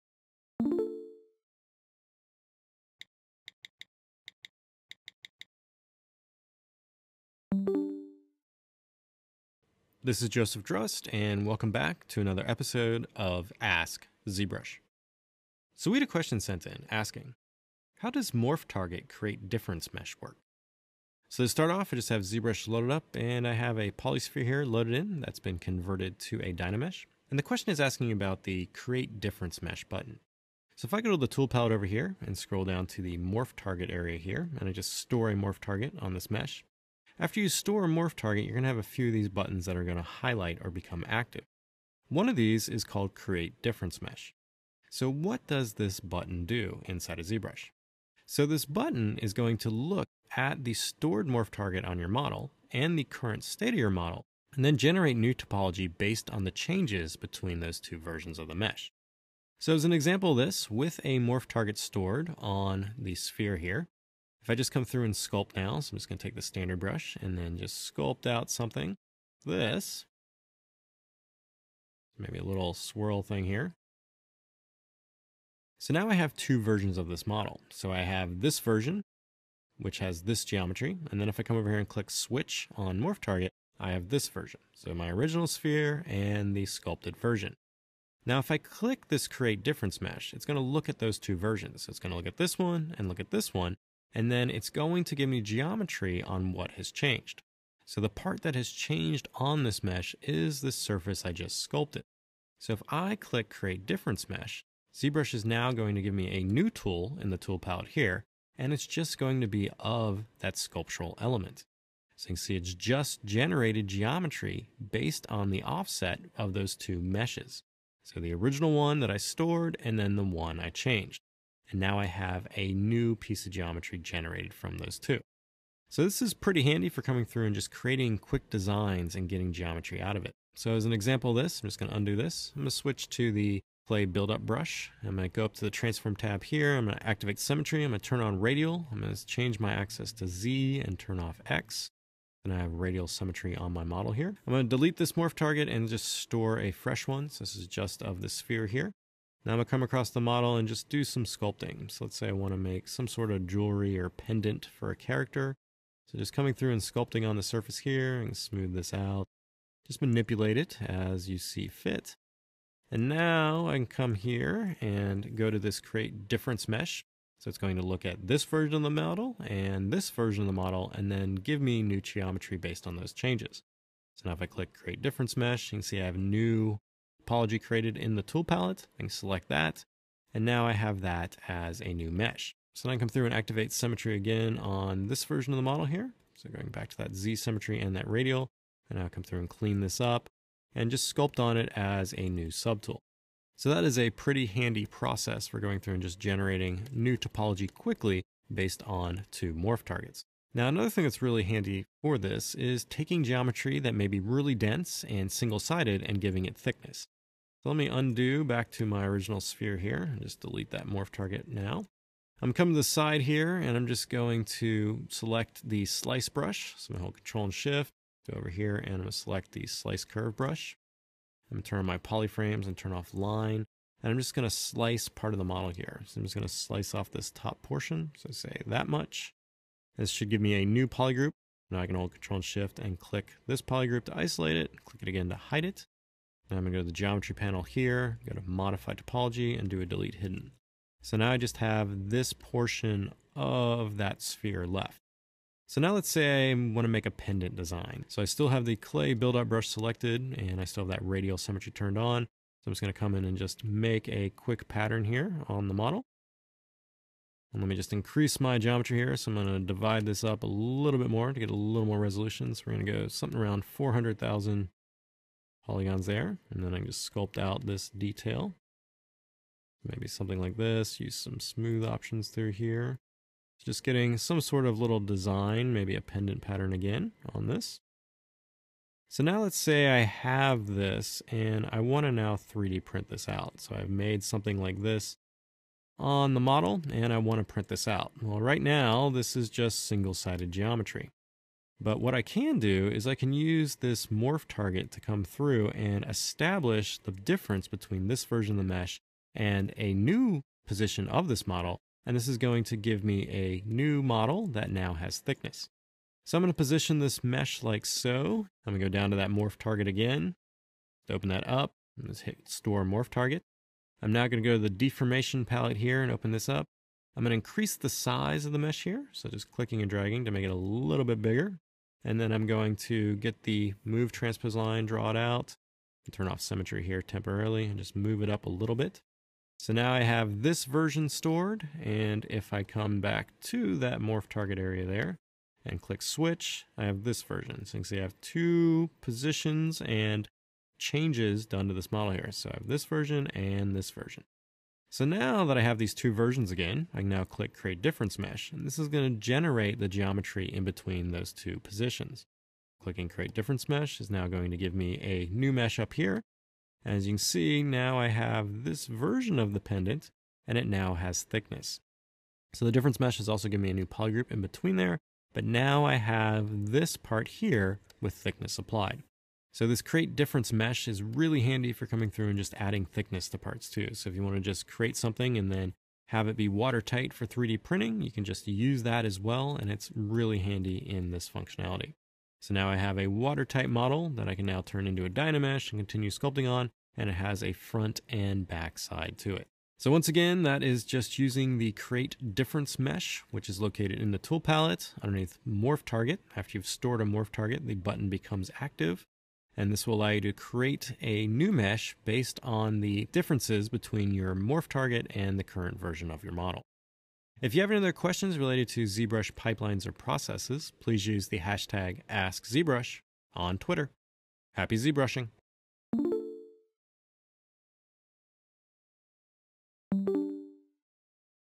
AskZBrush: “How does Morph Target - Create Difference Mesh work?”
Video covers how the Morph Target Create Difference functionality works inside of ZBrush.